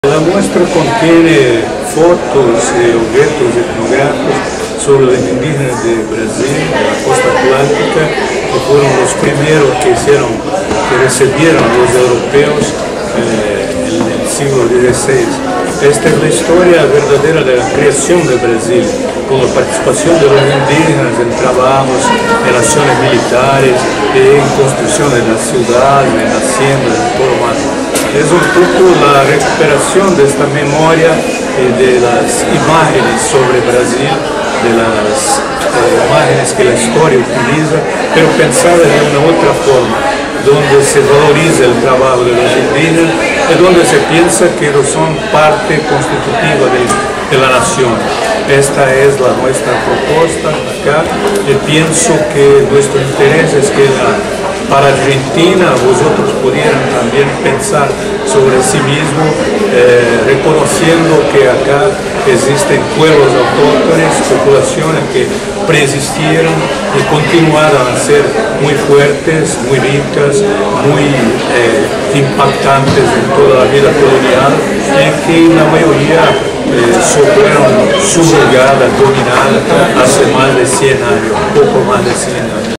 Joao Pacheco de Oliveira habla sobre la muestra "Los primeros brasileños"
Organizada en el marco de la décima Reunión de Antropología del MERCOSUR, "Los Primeros brasileños" es la muestra temporal que estuvo del 10 de julio hasta el 13 de octubre de 2013 en el Museo de Bellas Artes Evita-Palacio Ferreyra.
La muestra propone un paseo por la historia del Brasil, con un recorrido visual sobre las distintas maneras en las cuales los indígenas de la costa atlántica y del interior de la región nordeste/este fueron descubiertos e incorporados al proceso de construcción nacional.
Tanto la concepción y curaduría, como la coordinación de la investigación estuvo a cargo de Joao Pacheco de Oliveira, antropólogo brasileño que se desempeña en el Museo Nacional de la Universidad de Río de Janeiro.